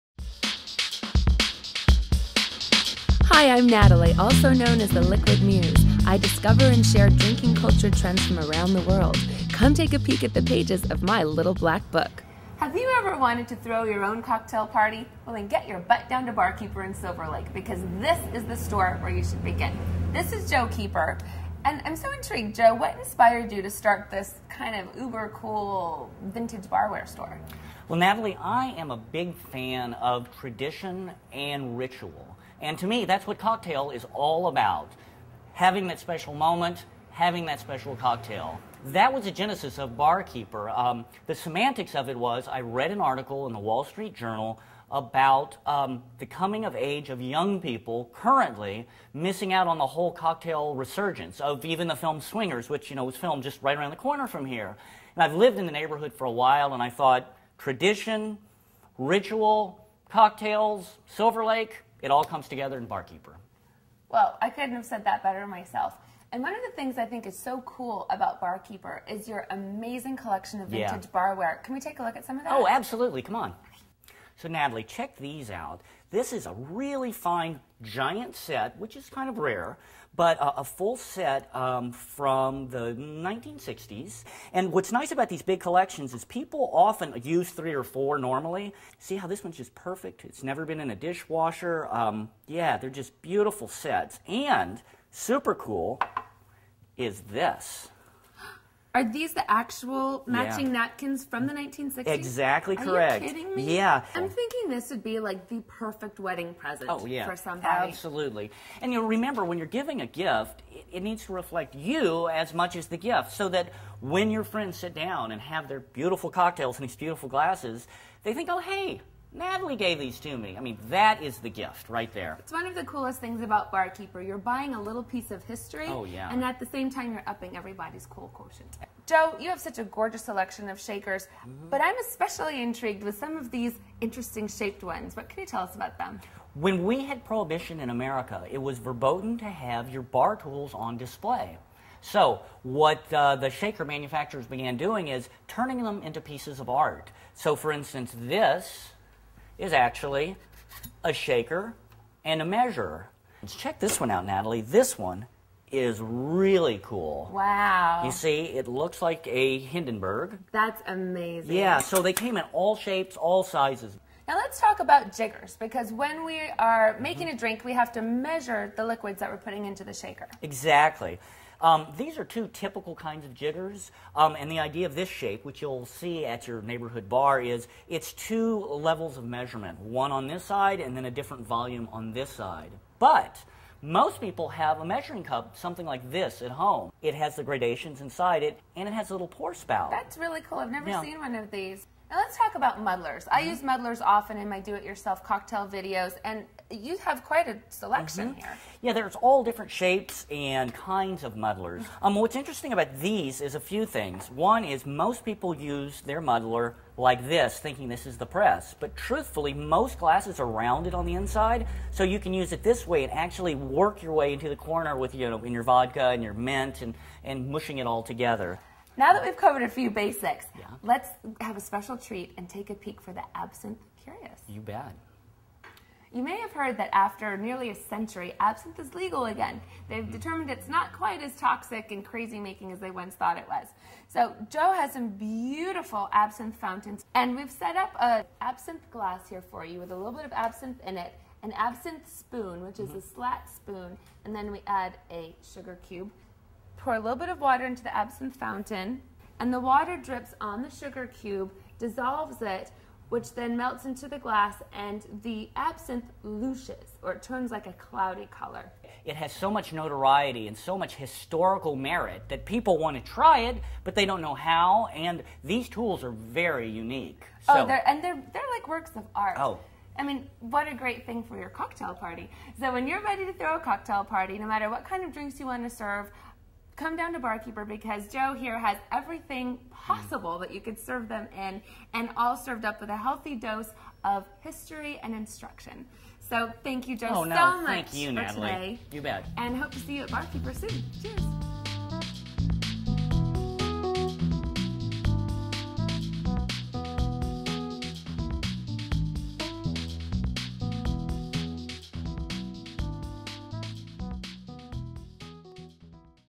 Pinky Vodka Presents: The Little Black Book Episode 2 Bar Keeper
Episode 2 of The Liquid Muse's Little Black Book series, presented by Pinky Vodka. The Little Black Book highlights drinking trends around the world. In this episode is the Bar Keeper the alcohol lover's paradise with such items as vintage barware, cocktail shakers and absinthe fountains. Anyone interested in throwing a home cocktail party can find their tools at the Bar Keeper in Silver Lake.